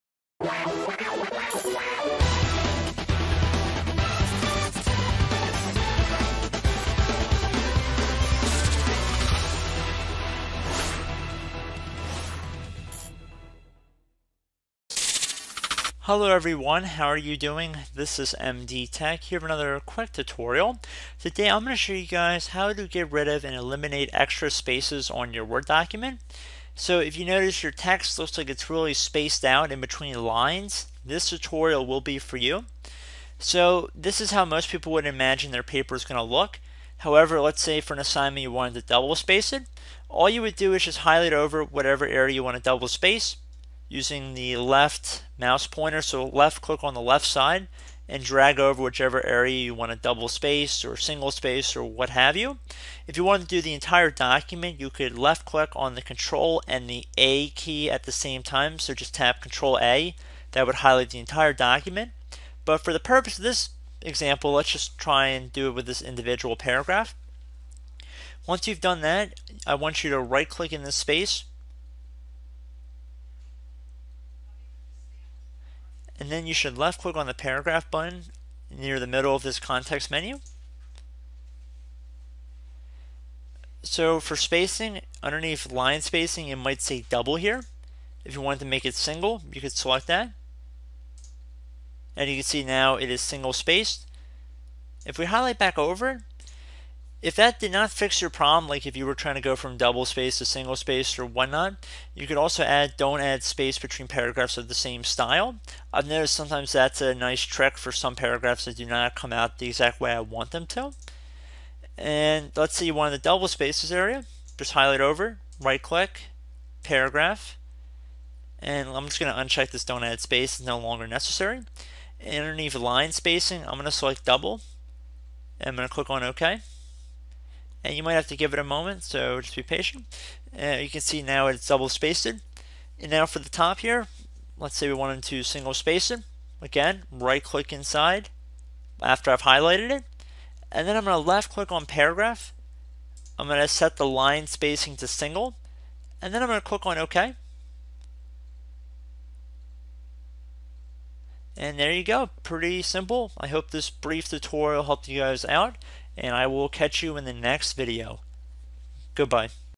Come eliminare spazi tra le righe Word?
Come rimuovere lo spazio extra tra le righe su un documento di Word.